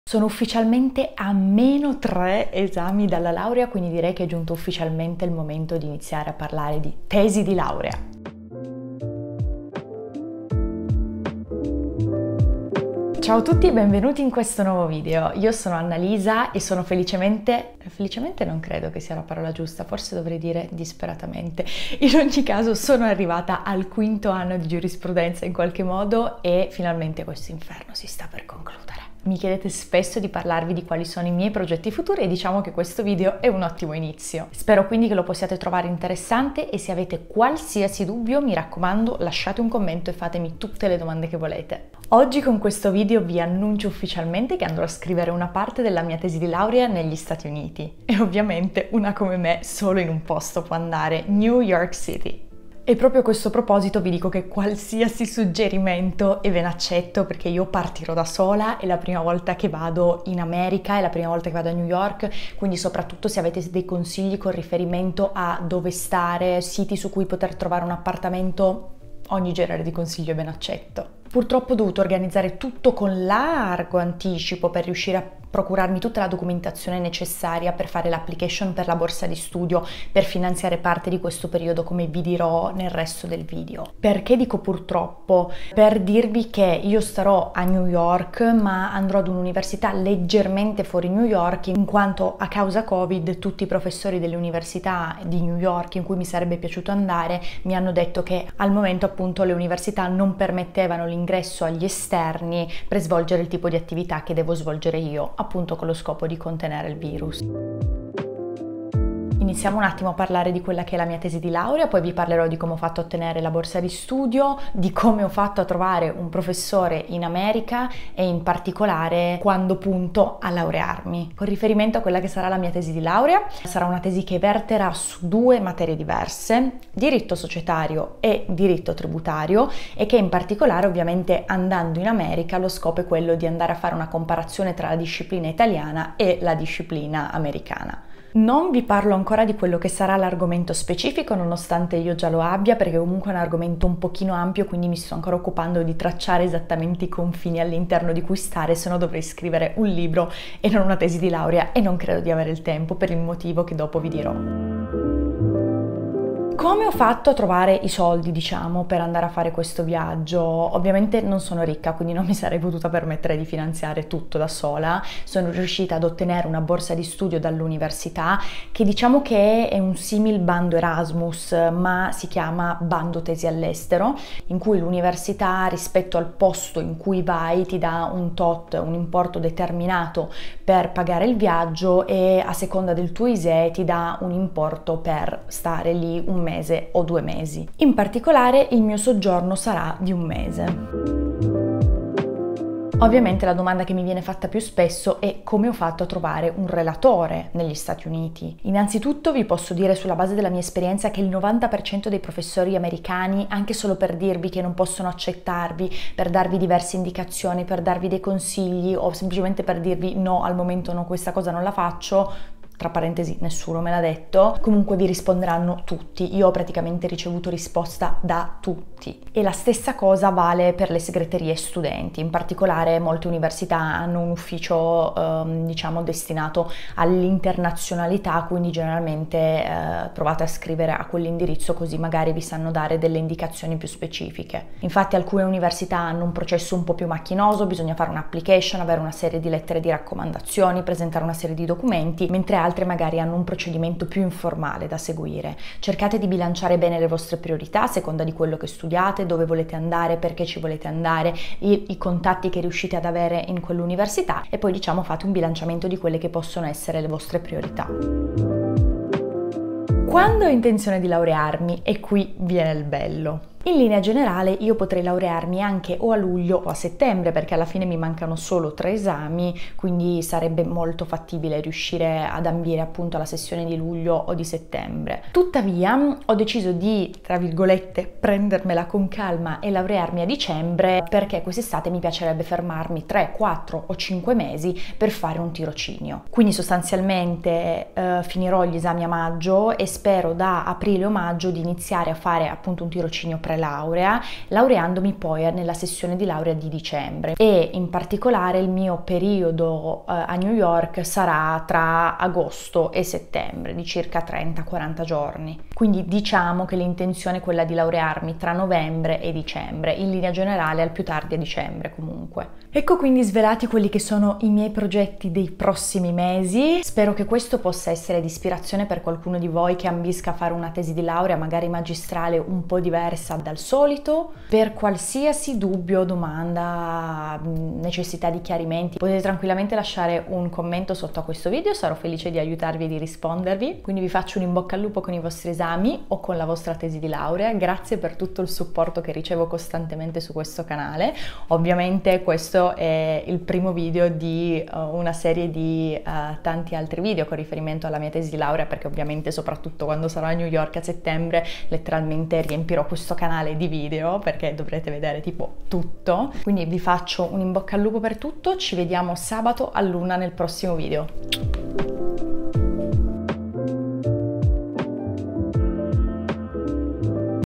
TESI DI LAUREA ALL'ESTERO: borse di studio, trovare relatore all'estero, quando mi laureo?
Ciao! Finalmente è ufficiale, vado a scrivere parte della mia tesi di laurea all'estero, guarda il video per scoprire come ho trovato un relatore all'estero e dove vado 🙊